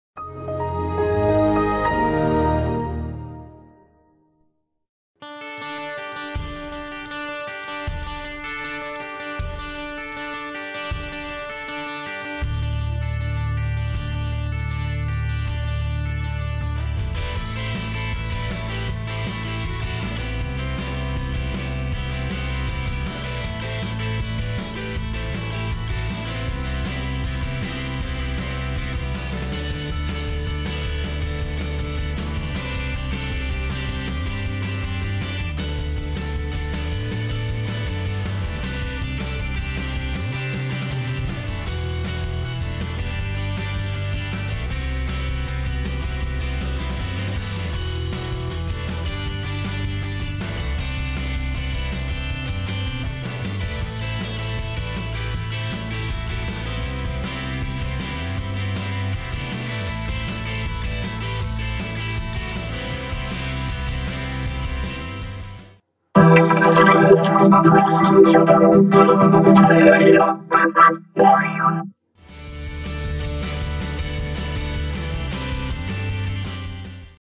How To Make G-Major 108 On Vegas Pro
Made on Monday, February 3, 2020, 10:32:01 pm to 10:32:58 pm

Let's learn how to make the original G-Major 108 on Vegas Pro.

Upload Time: February 7, 2020, 12:19 pm